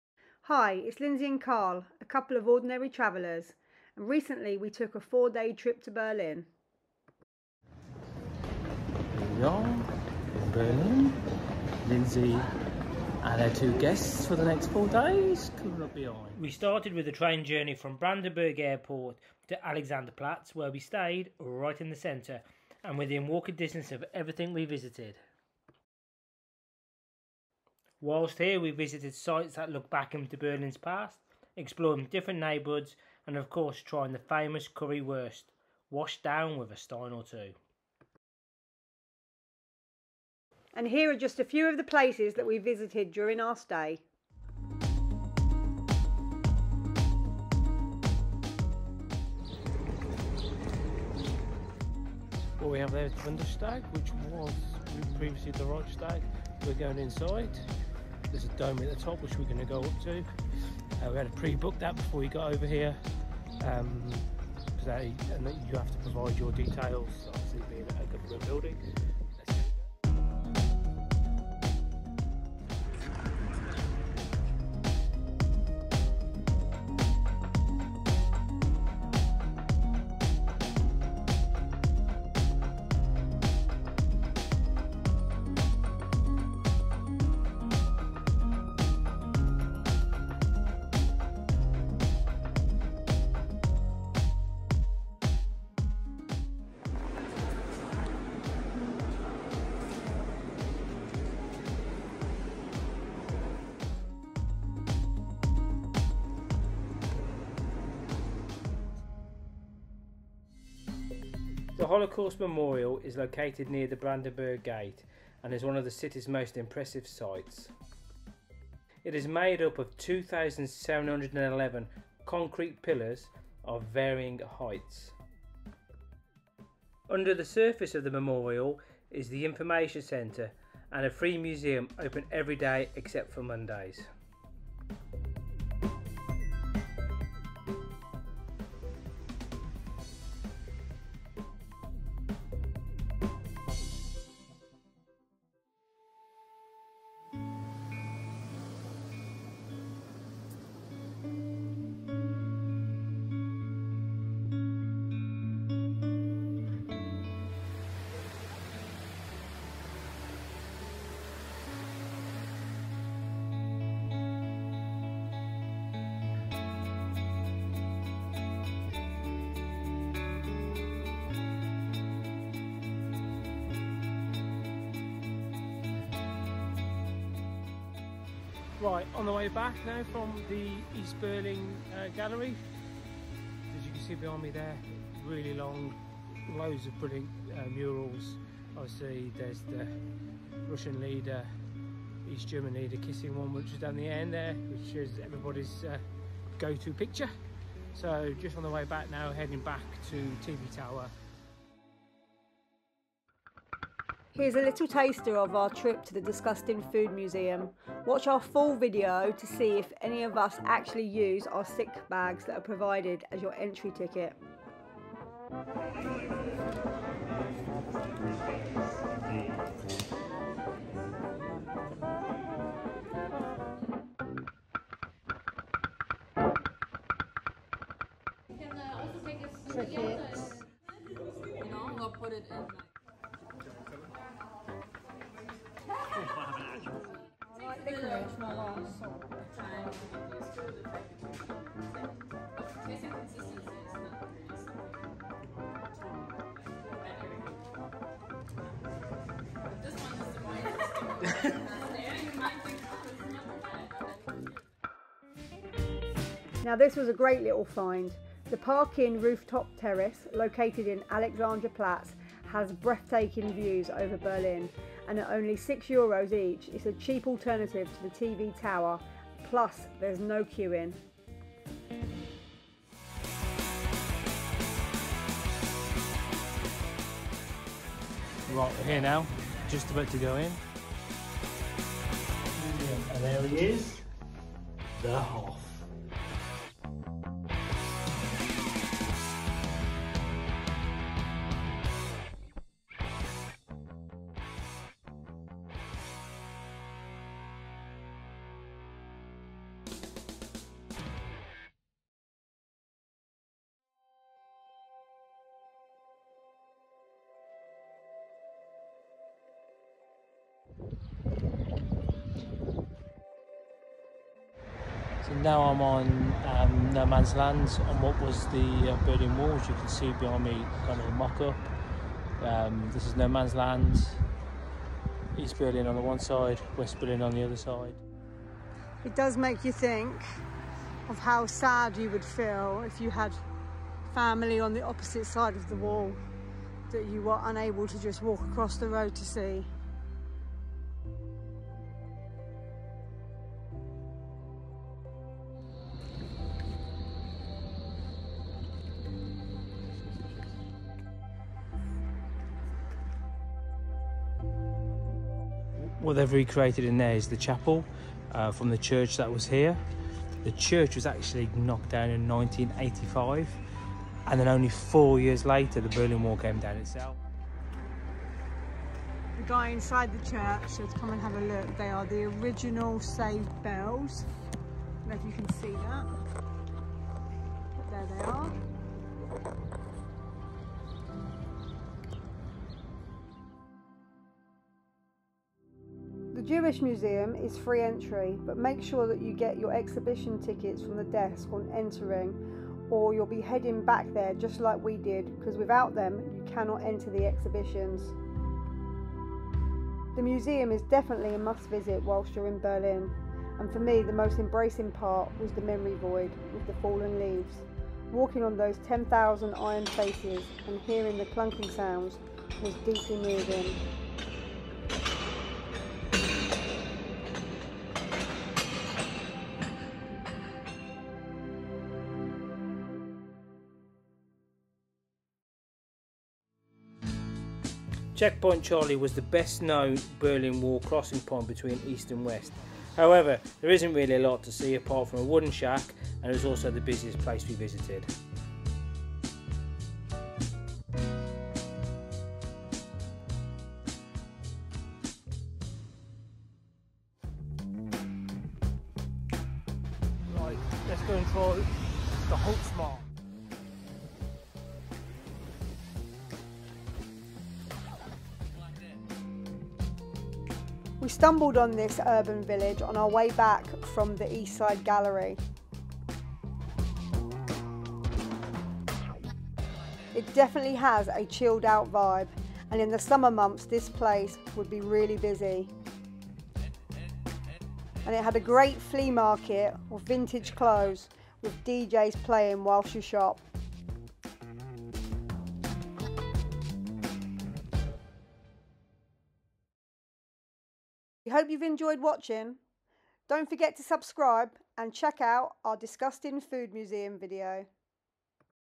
10 Things To Do In Berlin
🇩🇪 4 days in Berlin and we done loads. 🇩🇪
Here are 10 of the places we visited during our stay.
- Bundestag
- Memorial to the Murdered Jews
- East side Gallery
- Disgusting food Museum
- Park Inn Roof top Terrace
- David Hasselhoff Museum
- The Berlin Wall Memorial
- The Jewish Museum
- Checkpoint Charlie
- The Holzmarkt